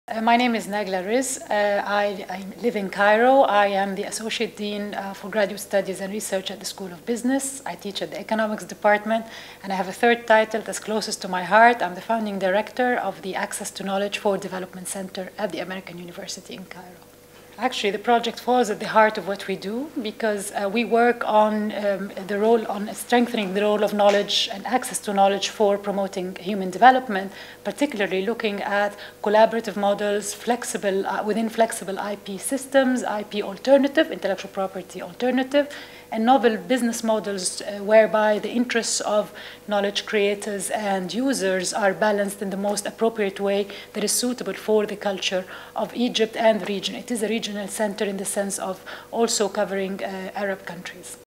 Open AIR's North Africa Hub - Prof Nagla Rizk - American University in Cairo
Prof. Nagla Rizk explains how Open AIR's objectives link to the work of the research unit she directs in Cairo: the Access to Knowledge for Development Center (A2K4D) at American University in Cairo. A2K4D is the Open A.I.R. network's North Africa Hub, and Rizk and her colleagues at the Center have coordinated two Open AIR case studies.